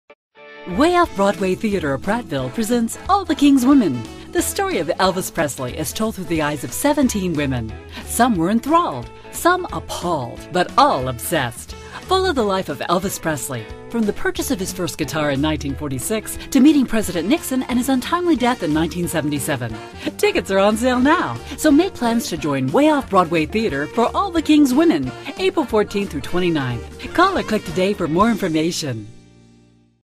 Way Off Broadway Theatre presents "All the King's Women"
The Way Off Broadway Theatre, located in downtown Prattville, is proud to announce the opening of their latest play, "All The King's Women", Saturday, April 14. The play will be in production on Friday and Saturdays at 7:30 pm and Sundays at 2:00 pm through April 29, 2012.

This southern comedy tells the story of Elvis Presley through the eyes of seventeen women. Some of the women are enthralled, some are appalled, but all are OBSESSED!

"I am excited to bring Luigi Jannuzzi's All the King's Women to Prattville's Way Off Broadway Theatre stage," said Laura Morrison, director of the play. "Audiences will witness the impact Elvis Presley had on these women and his everlasting affect on popular culture."

Tickets can be purchased at the Cultural Arts Center, or by phone at . Tickets are $8 in advance or $10 at the door. To learn more about "All the King's Women", visit our Way Off Broadway Theatre page on this website.